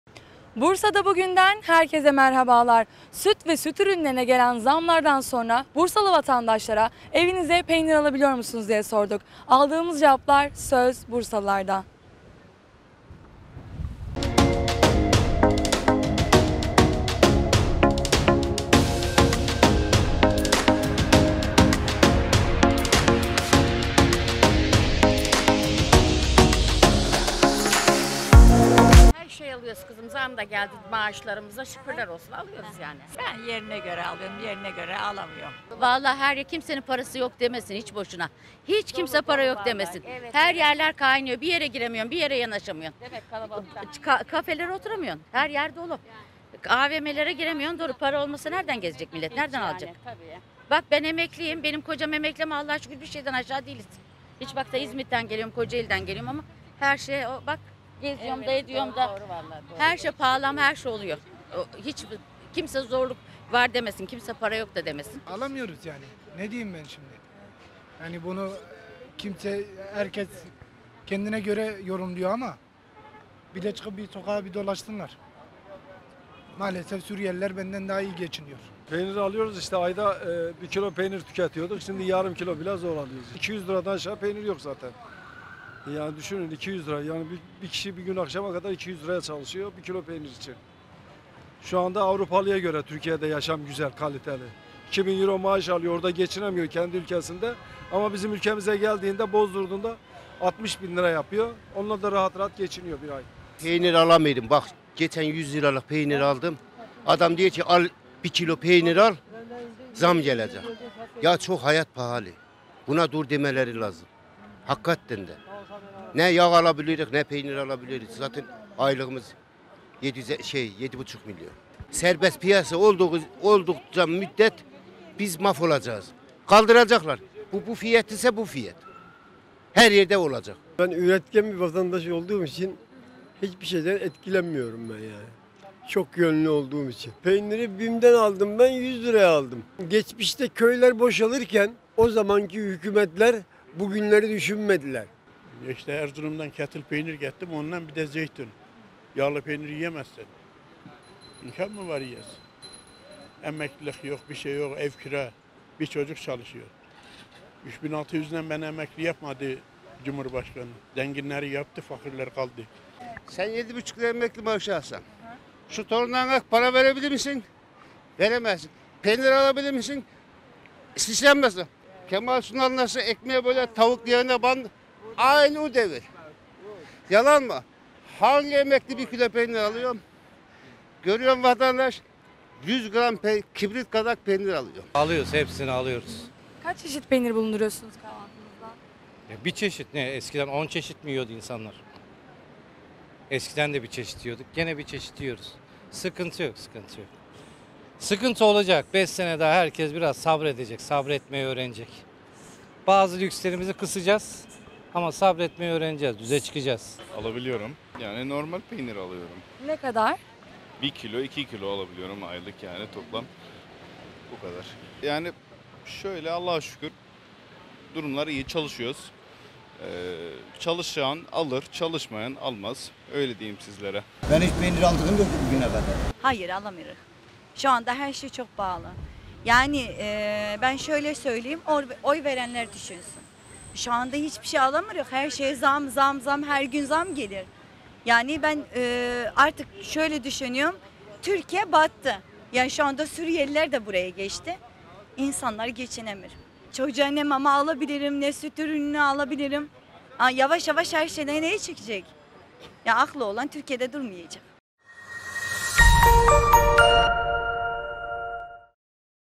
Bursalılara sorduk... Evinize peynir alabiliyor musunuz?
Söz Bursalılarda programında vatandaşlara, "Evinize peynir alabiliyor musunuz?" sorusunu yönelttik. İşte aldığımız cevaplar. Söz Bursalılarda...